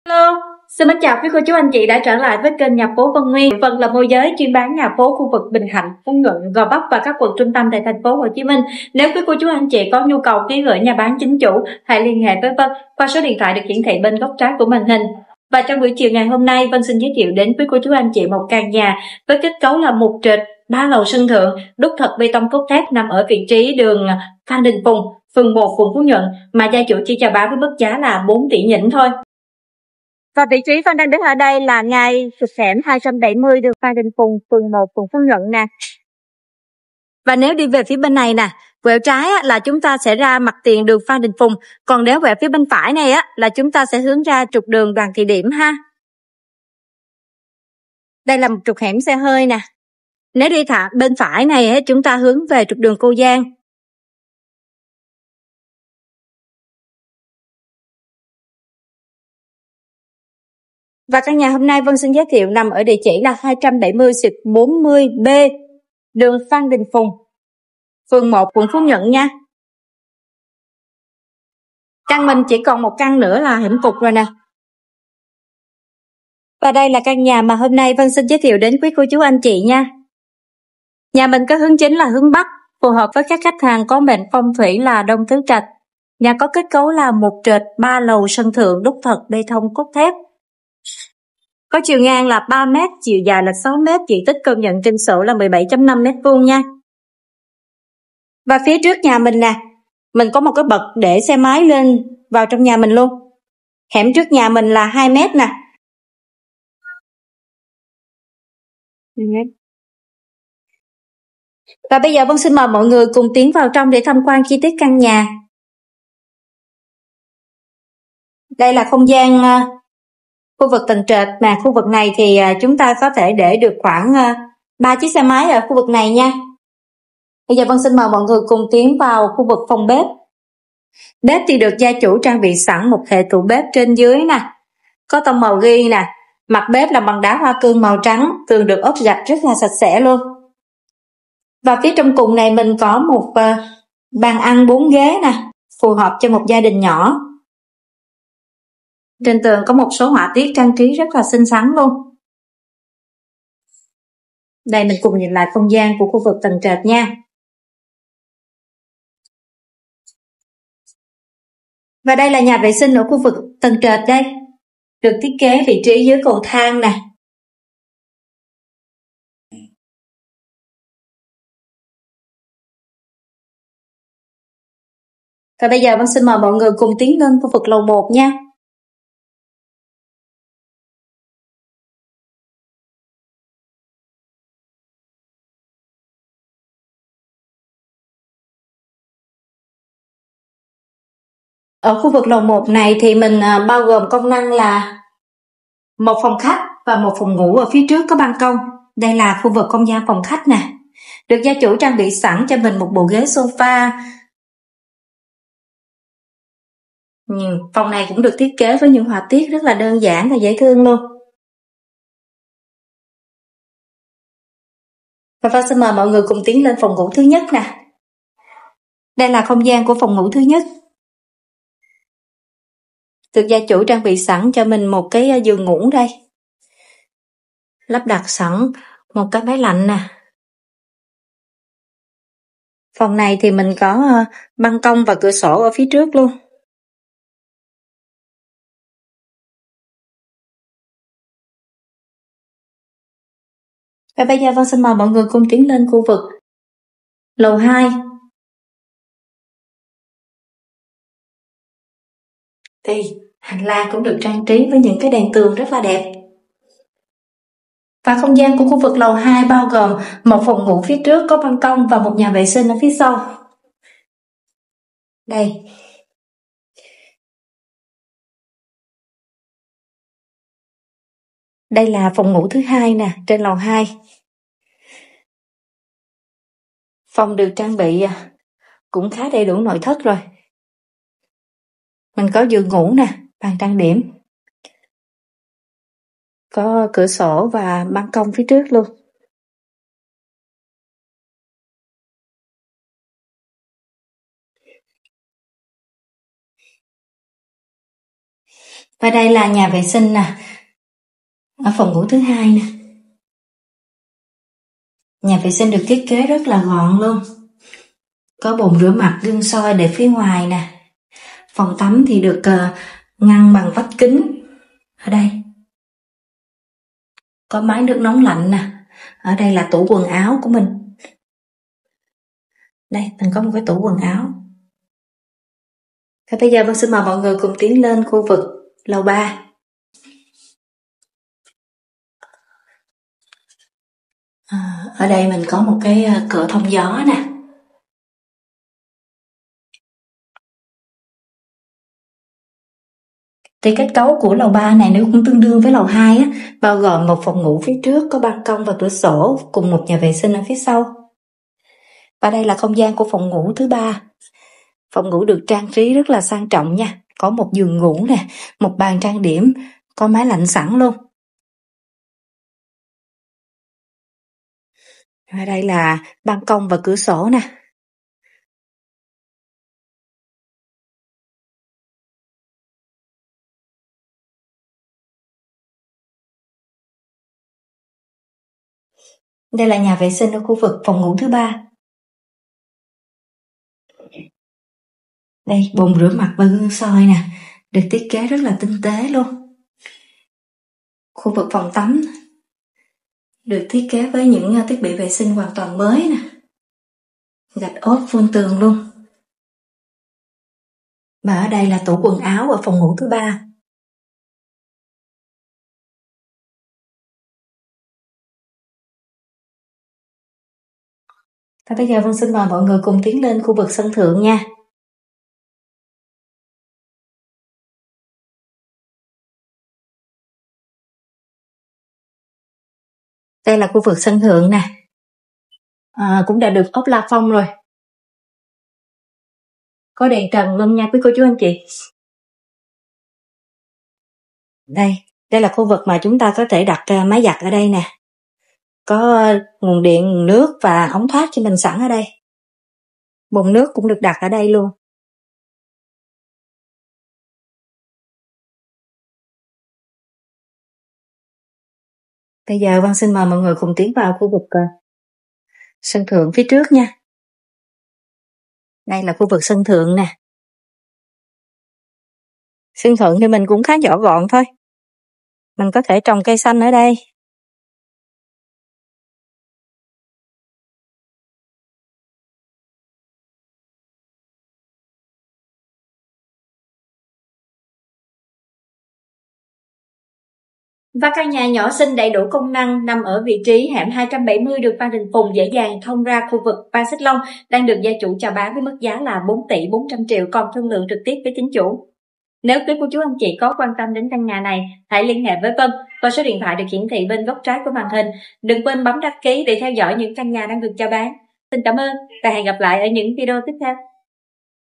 (Đã bán) 4.4 tỷ | Bán nhà Phú Nhuận thông khu VIP Phan Xích Long | Phan Đình Phùng - Đoàn Thị Điểm
4.4 tỷ | Bán nhà Phú Nhuận thông khu VIP Phan Xích Long | Phan Đình Phùng - Đoàn Thị Điểm